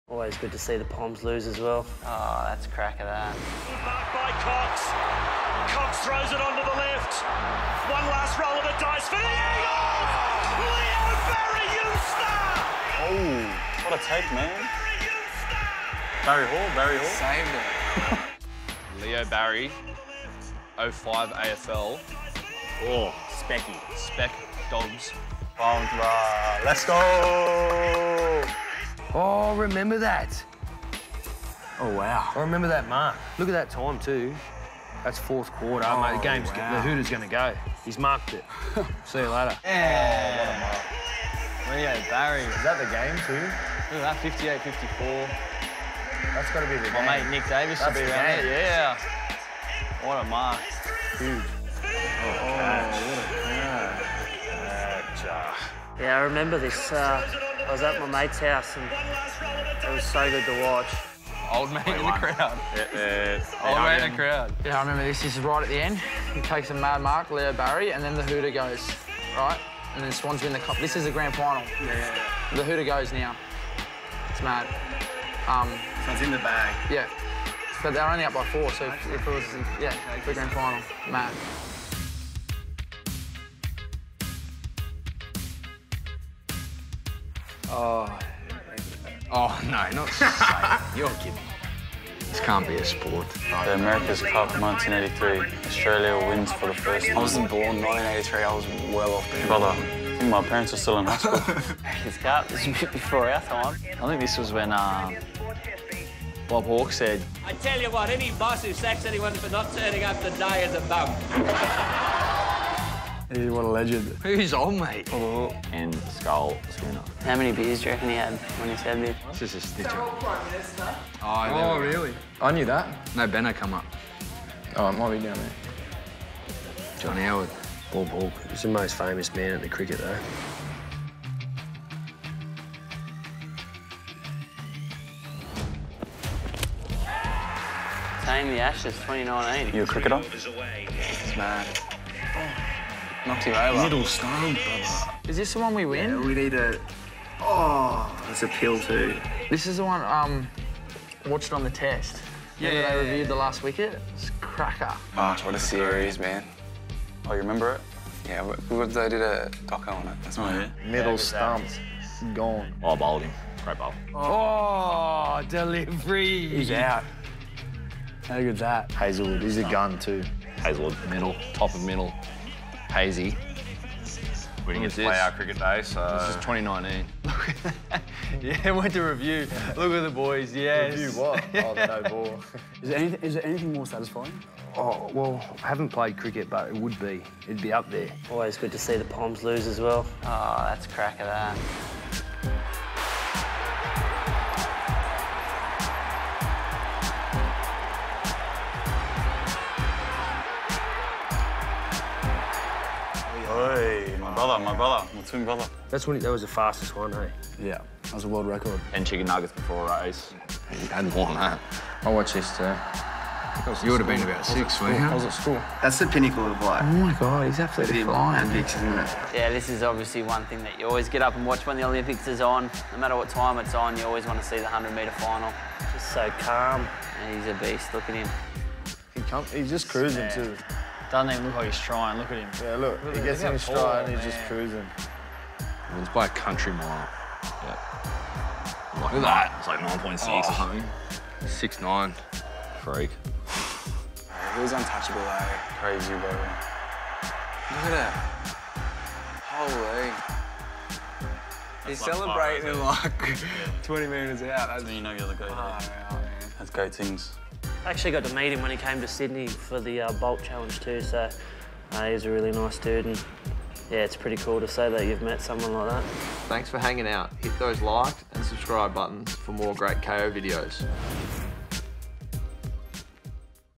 Footy players react | Epic moments in sport - part 2 | NRL | Kayo
👉 Stream every round of the NRL​​​ live and on-demand on Kayo.